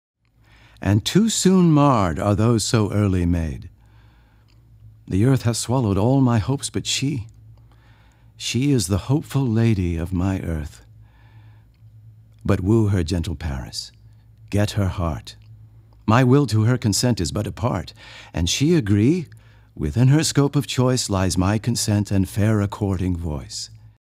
myShakespeare | Romeo & Juliet 1.2 Performance: Capulet, Lines 13-19
Capulet: Charley Robinson